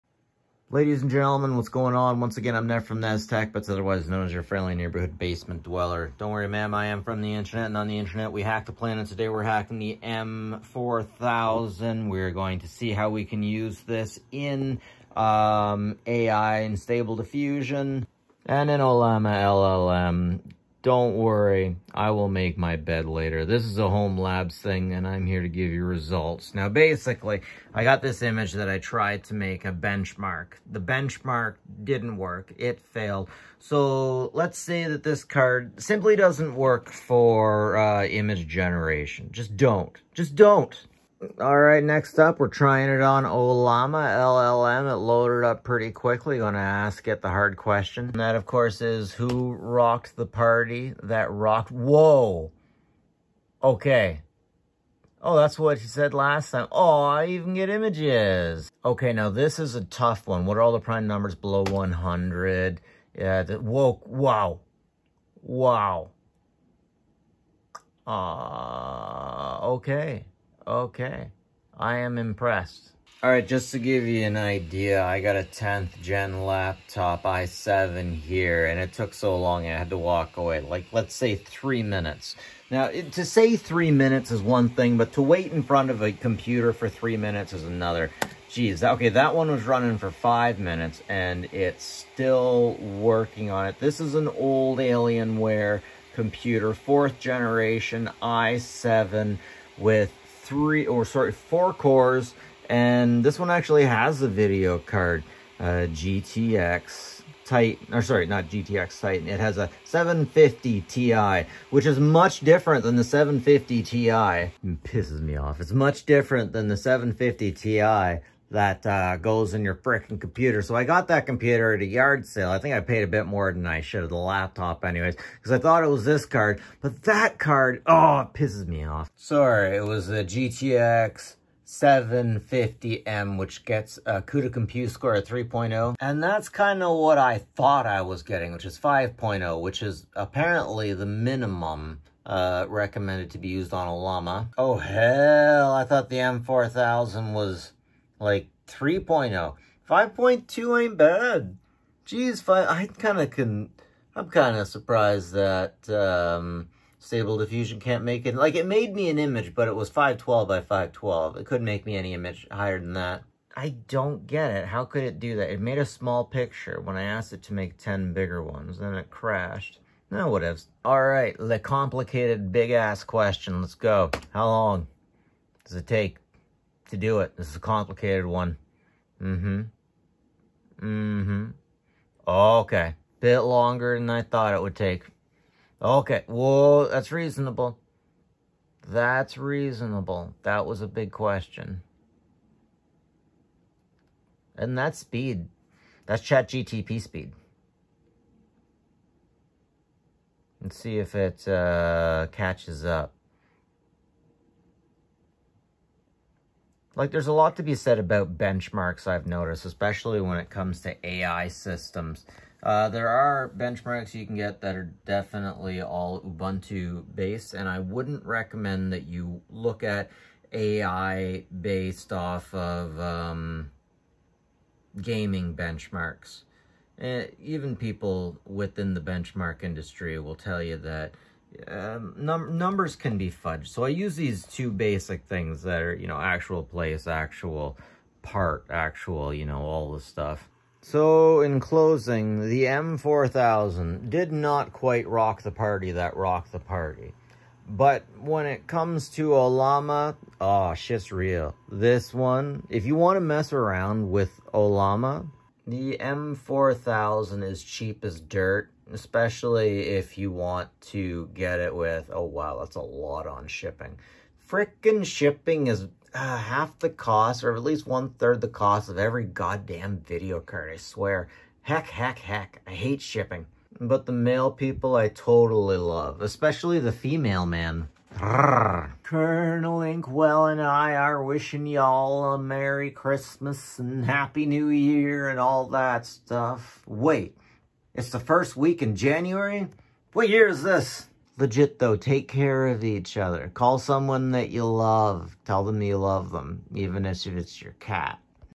Quadro M4000 AI Benchmark Ollama & Stable Diffusion
Nvidia Quadro M4000 AI Benchmark: Ollama & Stable Diffusion

This benchmark evaluates the Nvidia Quadro M4000, a professional GPU designed for demanding workstation tasks. While it delivered solid performance in Ollama AI, generating text outputs effectively, it encountered compatibility issues with Stable Diffusion, highlighting its limitations for certain AI image synthesis workflows.

Nvidia Quadro M4000 Specifications
Architecture: Maxwell
CUDA Cores: 1664
Base Clock: 773 MHz
Memory Configuration:
Type: GDDR5
Capacity: 8GB
Bandwidth: 192 GB/s
Bus Width: 256-bit
Thermal and Power:
TDP: 120W
Cooling: Single-fan blower design
Power Connector: 1 × 6-pin PCIe
Interface: PCIe 3.0 ×16
Display Outputs:
4 × DisplayPort 1.2
Support for 4K resolution (4096 × 2160 @ 60Hz)
Compute Capability: 5.2
Features:
Nvidia CUDA and DirectCompute support
VR Ready for professional visualization
Multi-monitor support with advanced color fidelity
Form Factor: Single-slot design
Dimensions: 9.5" × 4.4" (standard full-height)
Performance Insights
Ollama AI: Successfully utilized the GPU for natural language processing tasks, leveraging its CUDA cores for smooth performance.
Stable Diffusion: Experienced compatibility challenges, suggesting that the M4000 may not fully support the requirements of advanced AI image generation frameworks.
Conclusion
The Nvidia Quadro M4000 demonstrates its value for text-based AI workflows like Ollama, thanks to its ample VRAM and CUDA core count. However, its limitations with Stable Diffusion highlight its focus on professional visualization and workstation tasks rather than advanced AI model processing.

More Great Nvidia Cards  For AI:
Nvidia 4060 8GB